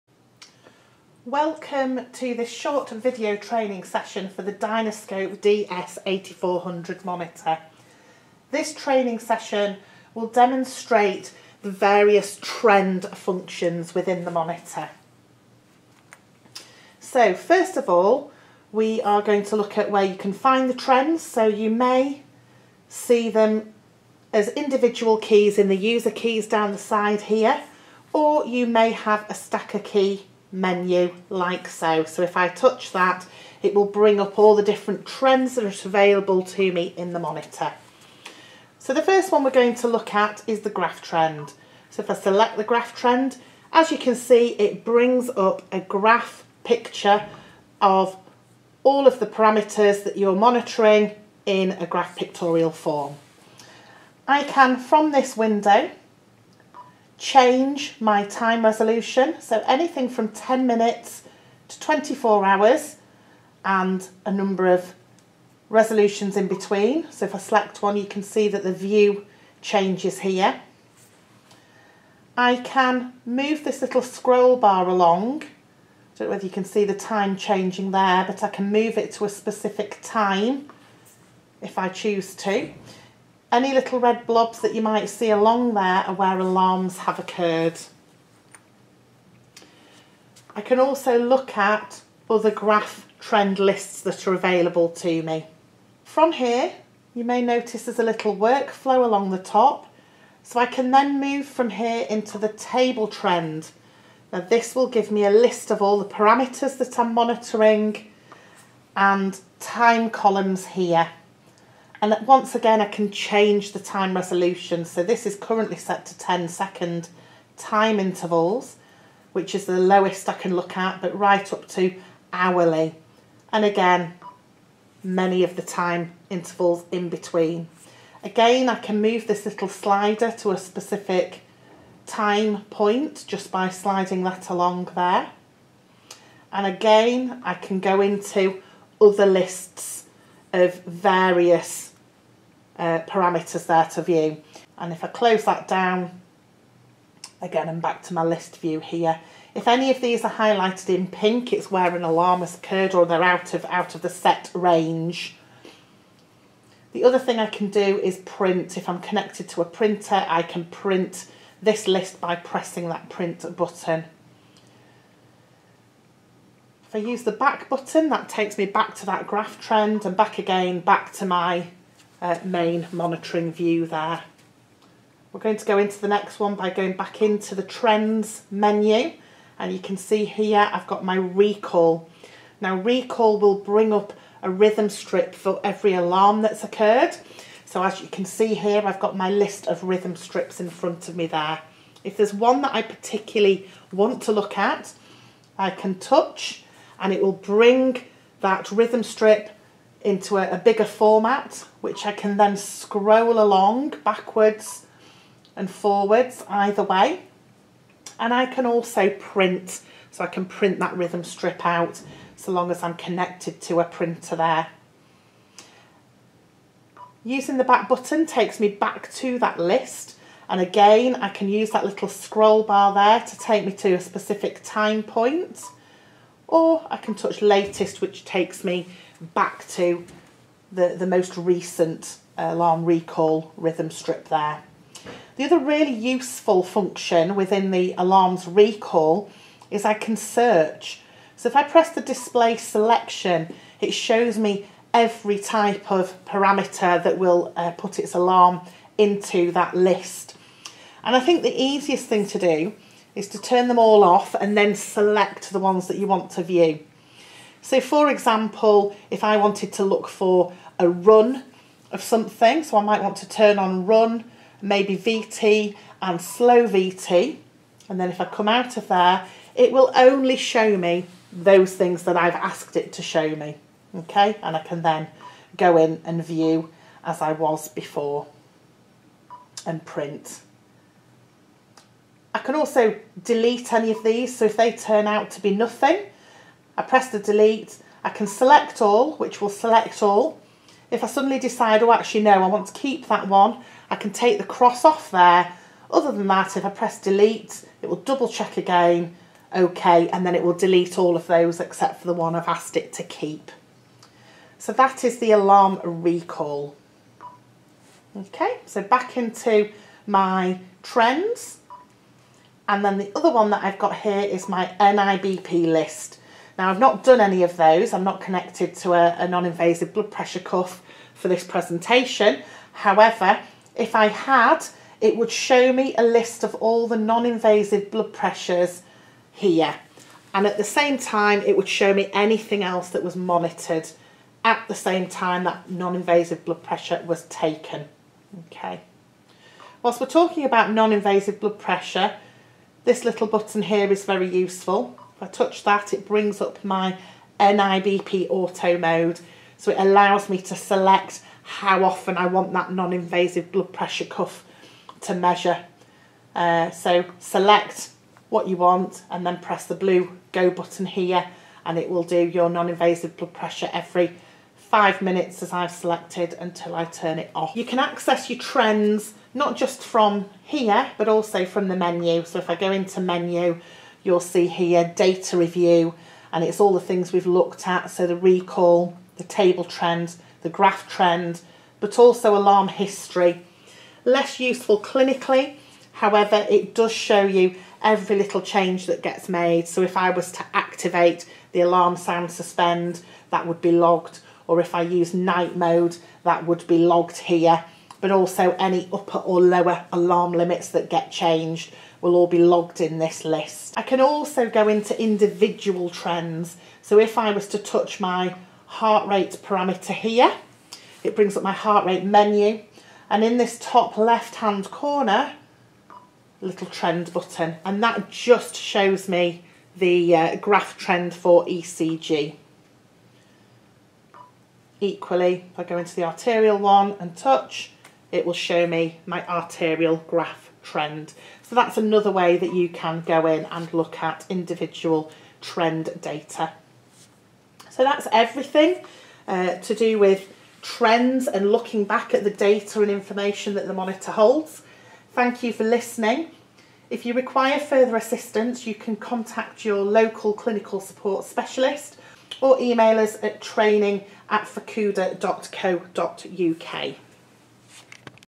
DS8400 Viewing Trends & Data
This short video will demonstrate and explain how to view your data and trends in the Dynascope DS-8400 Fukuda patient monitor.
- Alarm Recall
- Graph Trend
- Table Trend
- Alarm History